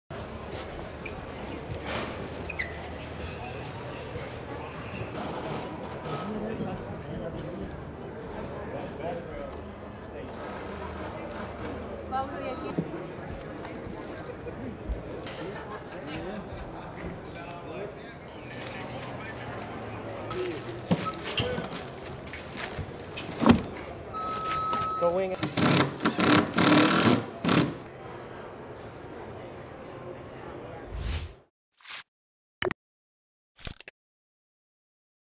السجادة الحمراء تنتظر نجوم هوليوود في حفل الأوسكار
ينهمك العمال في وضع اللمسات النهائية على السجادة الحمراء والديكور في هوليوود، وذلك استعداداً لحفل توزيع جوائز الأوسكار، الأحد.